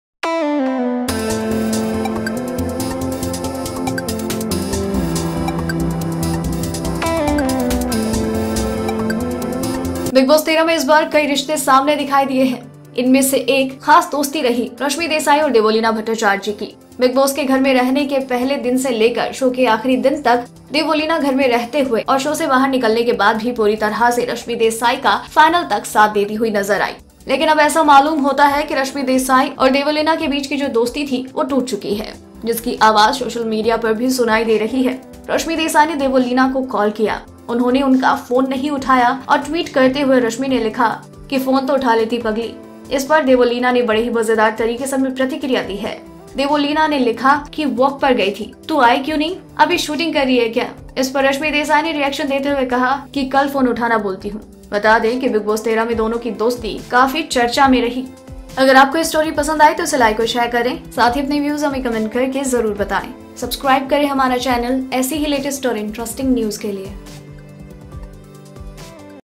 रश्मी देसाई और देवोलिना ने ट्विटर पर पर की एक-दुसरे की टांग खिचाई !!
Bollywood Planet ( BP ) : रश्मी देसाई और देवोलिना ने ट्विटर पर पर की एक-दुसरे की टांग खिचाई